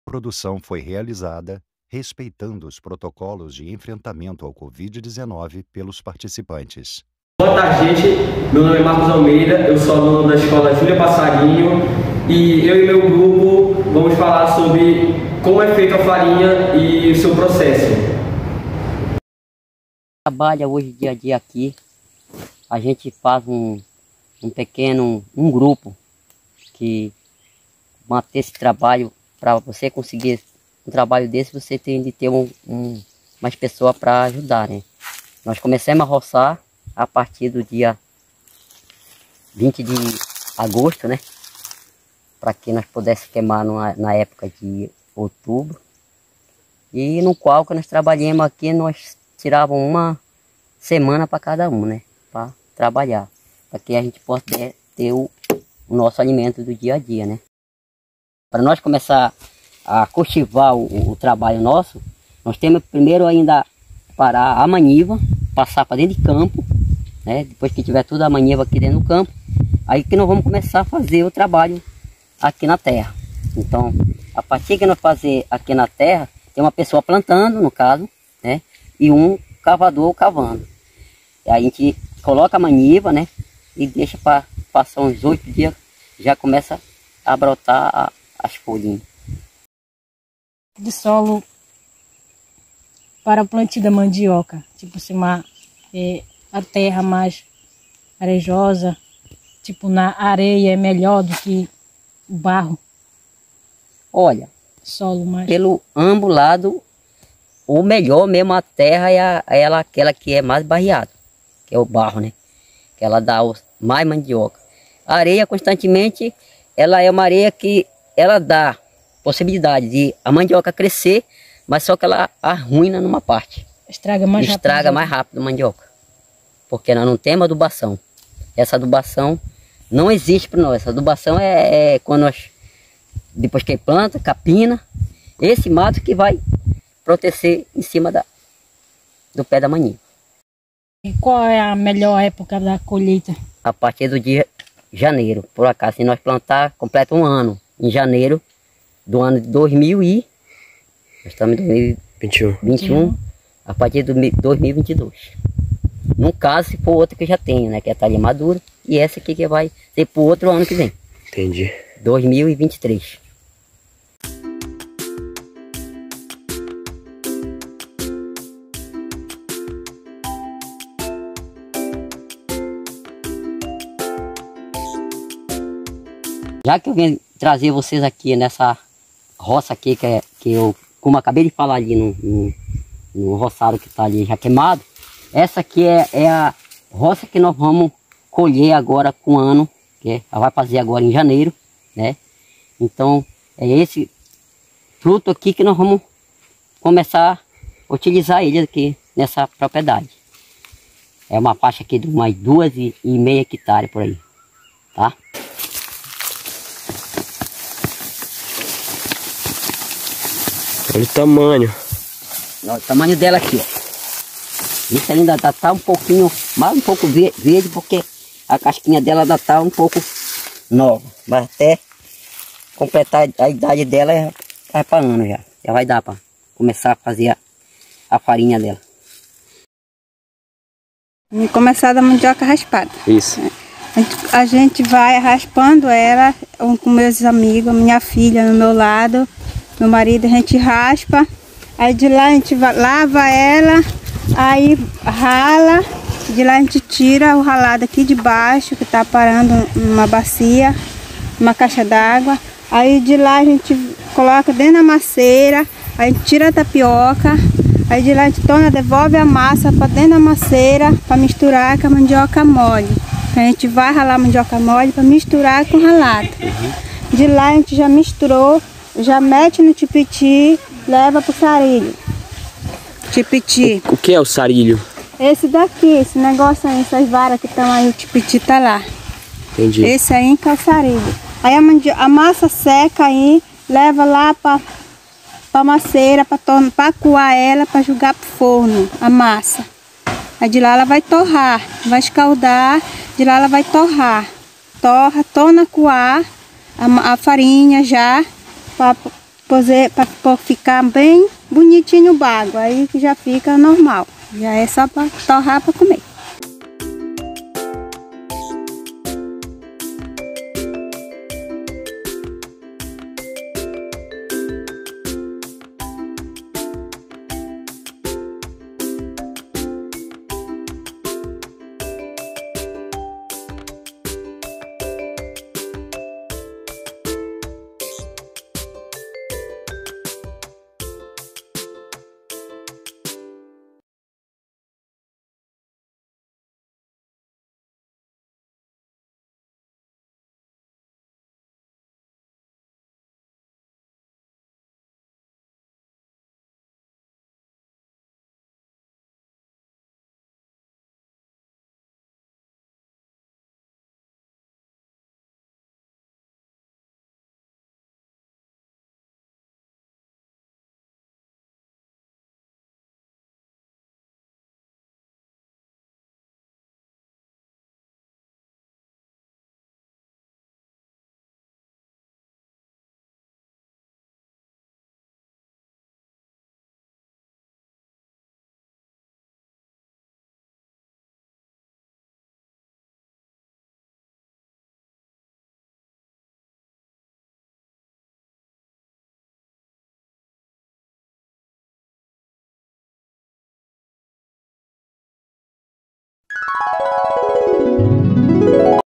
SNCT Pará 2021 - Webdocumentários Cientistas da Realidade - Processo de fabricação da farinha
Produção audiovisual realizada por estudantes da Escola Estadual Júlia Passarinho (Cametá-Pará), como parte da programação da SNCT-Pará 2021. O trabalho teve como proposta fazer investigações científicas em regiões ribeirinhas e rurais no município.

Neste webdocumentário, o grupo explora o processo de produção e fabricação da farinha de mandioca, desde o cultivo até o preparo.

Produção: Marcos, Vanussa, Mauro e Chayane
Participantes:  Marcos Almeida, Marcos Wender, Marcos Henrique, Vanussa, Jailson, Railson, Fábio, Adriano, Maria Erica, Maria de Cleofas e Matheus
Editor: Marcos Almeida